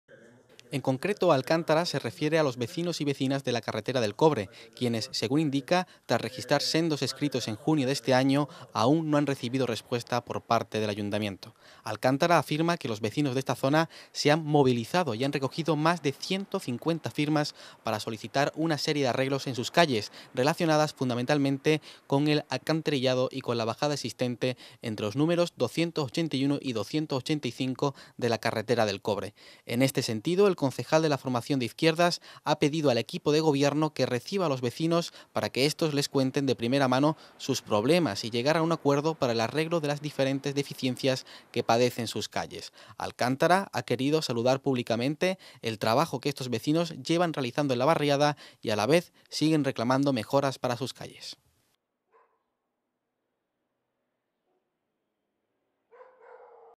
Izquierda Unida denuncia la falta de respuesta del alcalde a los vecinos de la carretera del Cobre
El concejal del grupo municipal de Izquierda Unida en el Ayuntamiento de Algeciras, José Luis Alcántara, ha denunciado la falta de respuesta del Alcalde de la ciudad, a las barriadas algecireñas.